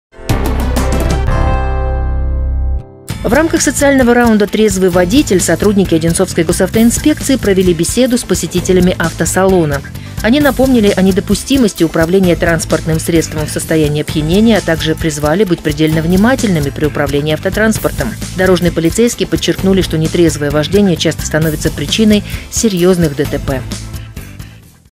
Трезвый водитель
В рамках социального раунда «Трезвый водитель» сотрудники Одинцовской Госавтоинспекции провели беседу с посетителями автосалона. Они напомнили о недопустимости управления транспортным средством в состоянии опьянения.